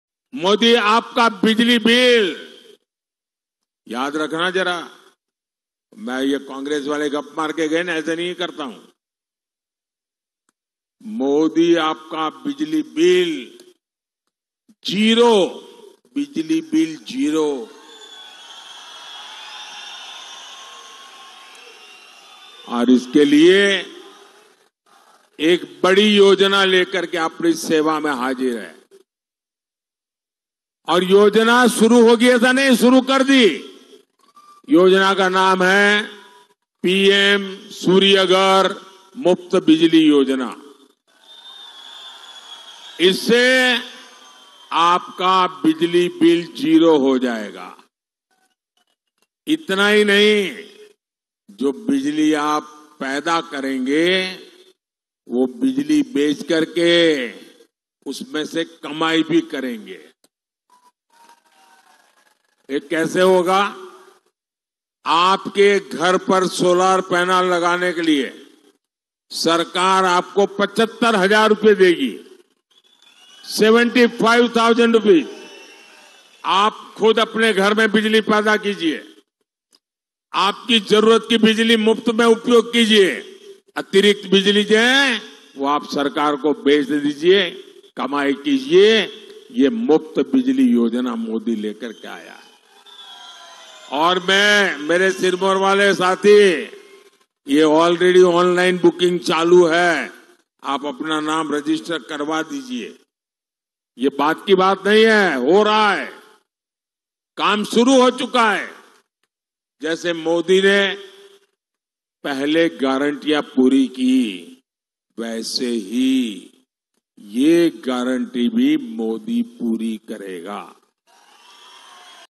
Muft Bijli Yojana | मंच से बोले पीएम मोदी 'मोदी आपका बिजली बिल याद रखना जरा…'
Incoming Search Terms: